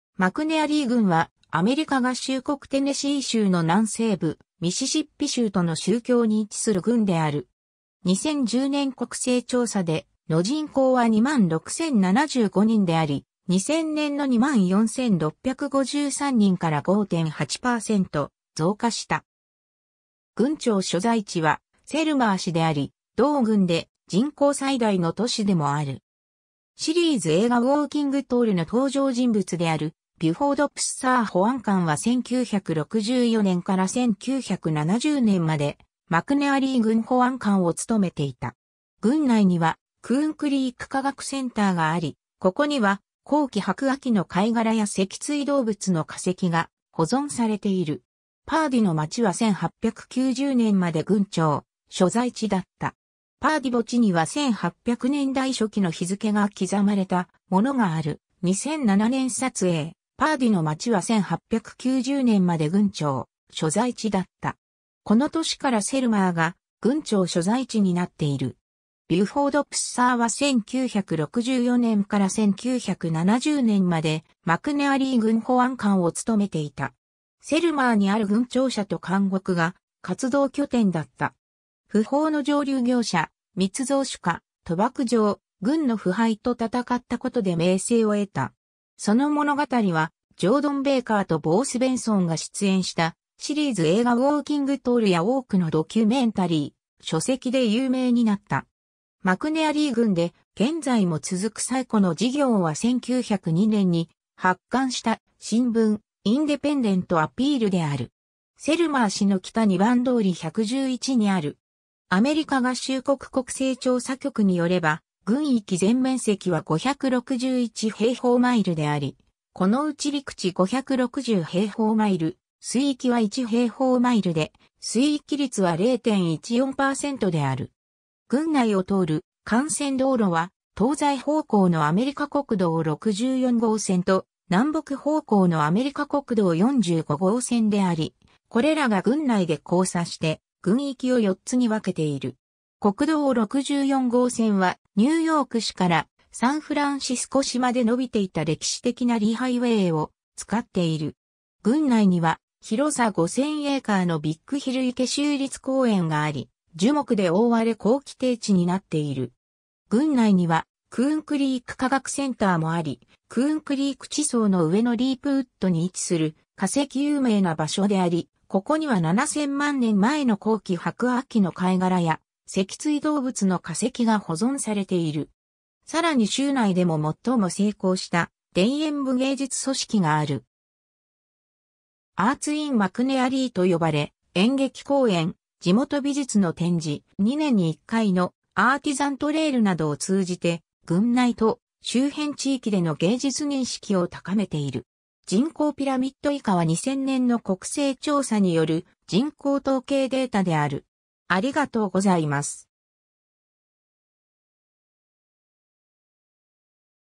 マクネアリー郡 (テネシー州)
マクネアリー郡（英: McNairy County）は、アメリカ合衆国テネシー州の南西部、ミシシッピ州との州境に位置する郡である。
2010年国勢調査での人口は26,075人であり、2000年の24,653人から5.8%増加した。
郡庁所在地はセルマー市（人口4,396人）であり、同郡で人口最大の都市でもある。
 シリーズ映画Walking Tall の登場人物であるビュフォード・プッサー保安官は1964年から1970年までマクネアリー郡保安官を務めていた。
 郡内にはクーン・クリーク科学センターがあり、ここには後期白亜紀の貝殻や脊椎動物（モササウルスなど）の化石が保存されている。
 パーディの町は1890年まで郡庁所在地だった。
パーディ墓地には1800年代初期の日付が刻まれたものがある、2007年撮影 パーディの町は1890年まで郡庁所在地だった。
この年からセルマーが郡庁所在地になっている。
 ビュフォード・プッサーは1964年から1970年までマクネアリー郡保安官を務めていた。
セルマーにある郡庁舎と監獄が活動拠点だった。
不法の蒸留業者、密造酒家、賭博場、郡の腐敗と戦ったことで名声を得た。
その物語は、ジョー・ドン・ベイカーとボー・スベンソンが出演したシリーズ映画Walking Tallや多くのドキュメンタリー、書籍で有名になった。
 マクネアリー郡で現在も続く最古の事業は1902年に発刊した新聞「インデペンデント・アピール」である。
セルマー市の北2番通り111にある。
 郡内を通る幹線道路は東西方向の アメリカ国道64号線と南北方向の アメリカ国道45号線であり、これらが郡内で交差して郡域を4つに分けている。
国道64号線はニューヨーク市からサンフランシスコ市まで伸びていた歴史的なリー・ハイウェイを使っている。
 郡内には広さ5,000エーカー (20 km²) のビッグヒル池州立公園があり、樹木で覆われ硬木低地になっている。
 郡内にはクーン・クリーク科学センターもあり、クーン・クリーク地層の上のリープウッドに位置する化石有名な場所であり、ここには7,000万年前の後期白亜紀の貝殻や脊椎動物（モササウルスなど）の化石が保存されている。
 さらに州内でも最も成功した田園部芸術組織がある。
アーツ・イン・マクネアリーと呼ばれ、演劇公演、地元美術の展示、2年に1回のアーティザン・トレイルなどを通じて郡内と周辺地域での芸術認識を高めている。